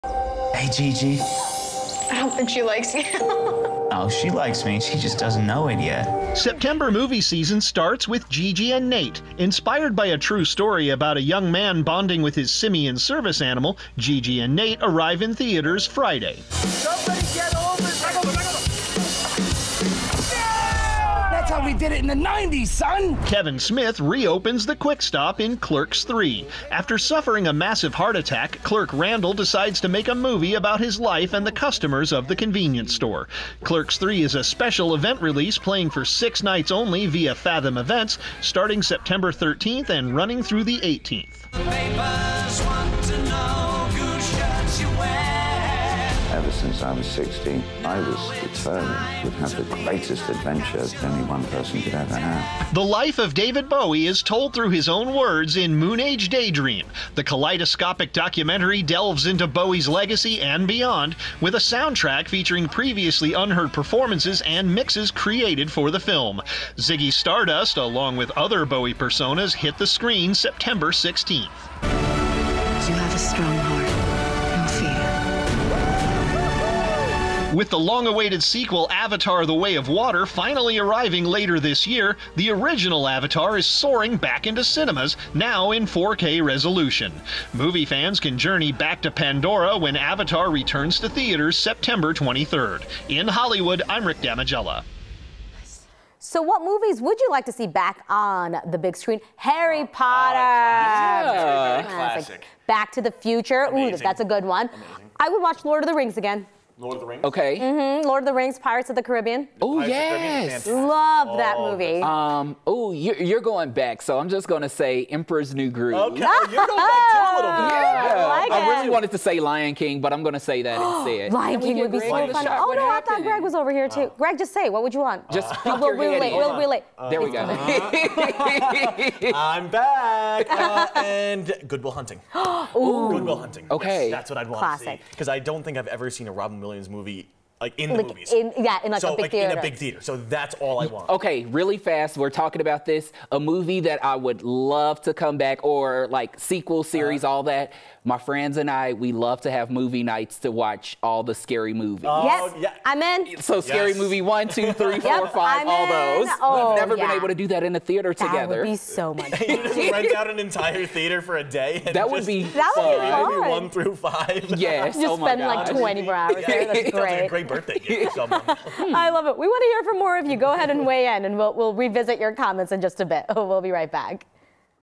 Movies heading to theaters in September 2022
A sequel to "Clerks," a David Bowie documentary and the return of "Avatar" are just some of the movies heading to theaters in September.

9NEWS (KUSA) is located in Denver, Colorado.